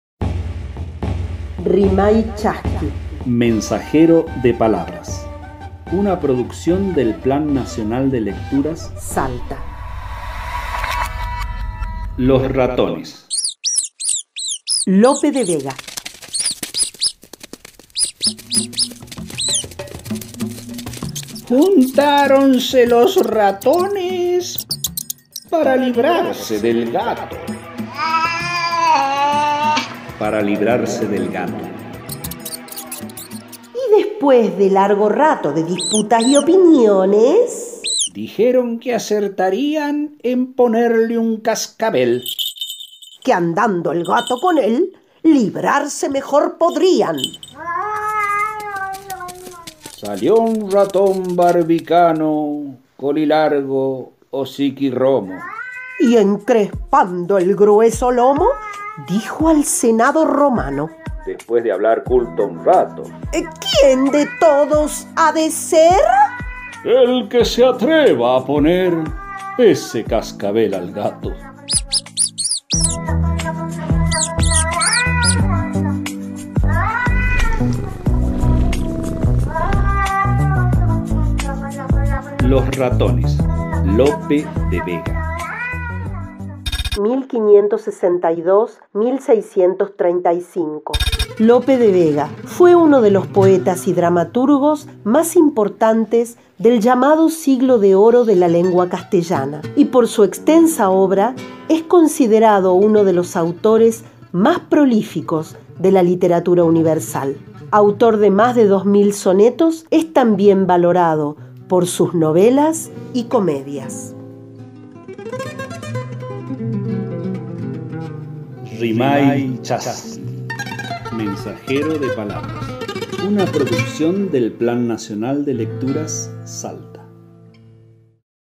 Rimay Chasqui/ Los ratones - Lope de Vega
Los Ratones
Lope de Vega es un referente de lo que se considera el Siglo de Oro español, sus comedias y obras teatrales renovaron las artes escénicas y literarias.

Rimay Chasqui se encuentra con una de estas piezas humorísticas y la trae para compartir, además,  viene con unos pequeños acompañantes y sus ideas revolucionarias.

Texto: Los Ratones

Autor: Lope de Vega

Voces: Claudia Peña y Fernando Arancibia (La Faranda)

Edición: Majo Bustamante

Realización: Plan Provincial de Lectura Salta